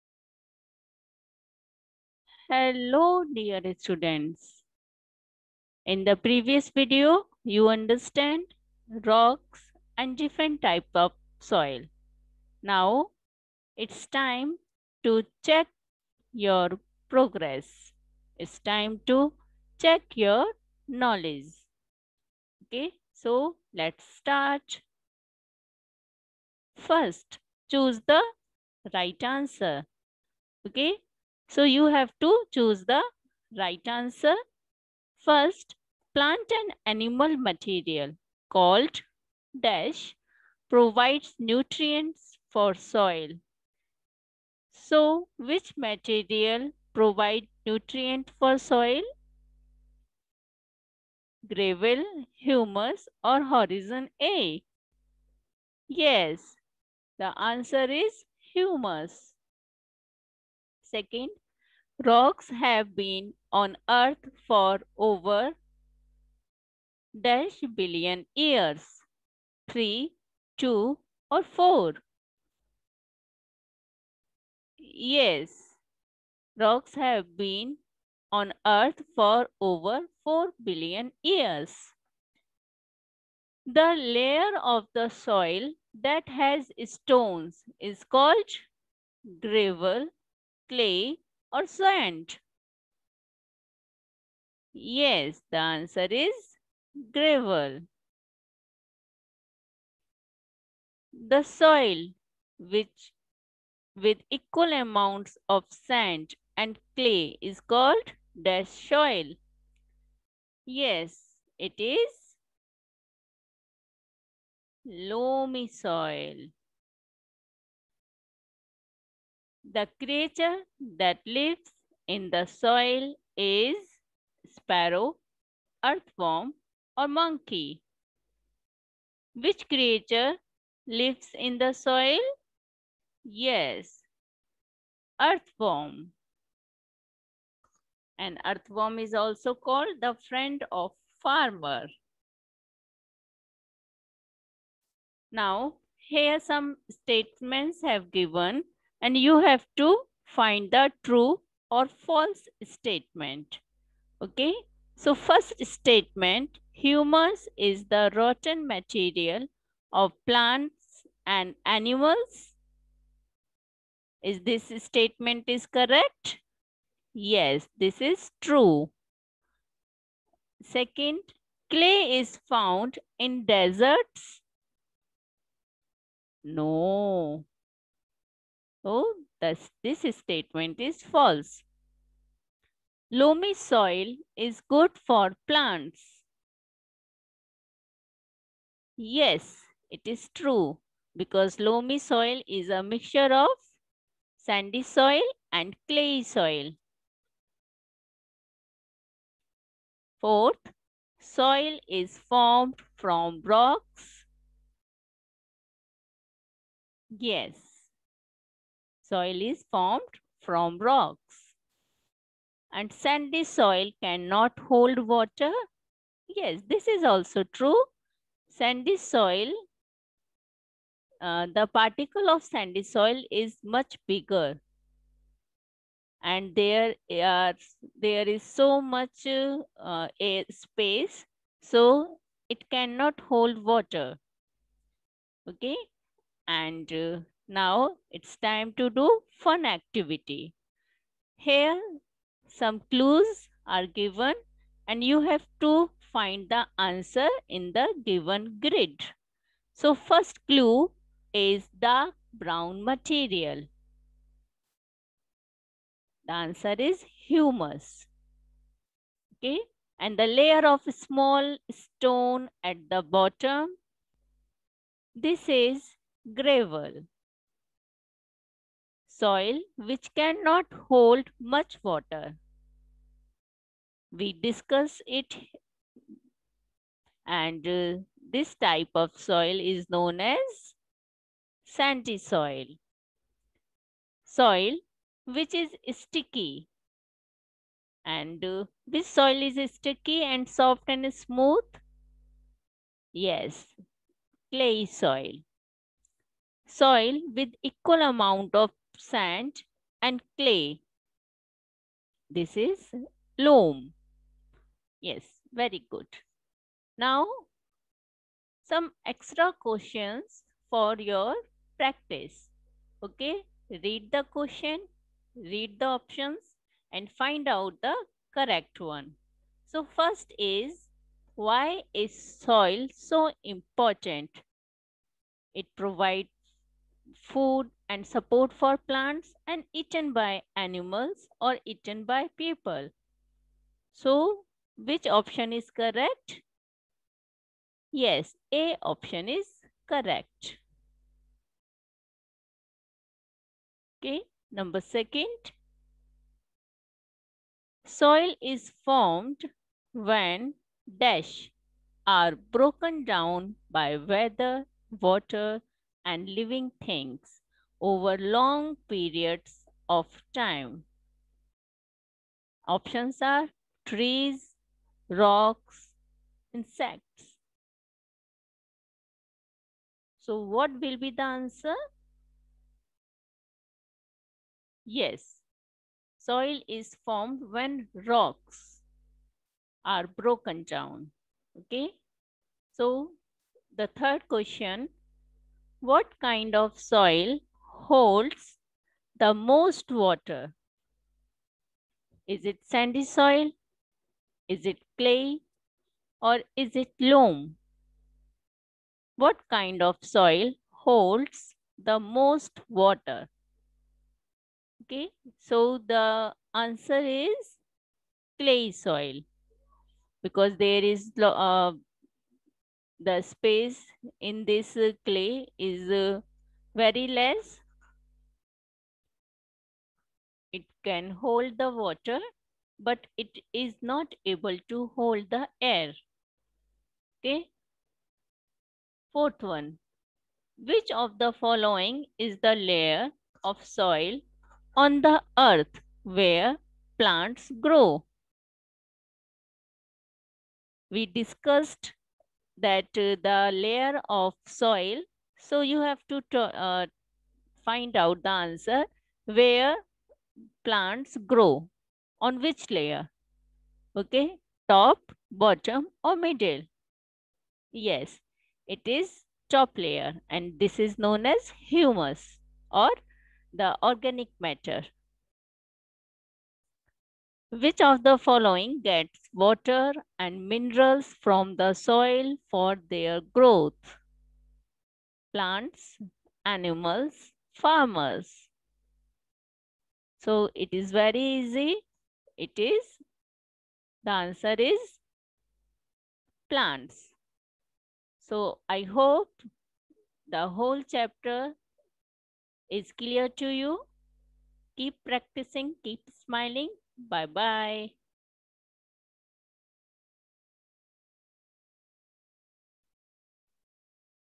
Rocks and soil class 3(check your progress)
Exercise-based on Rocks and soil.
-Choose the right answer.
-True/False
-Fun Activity
-Extra MCQ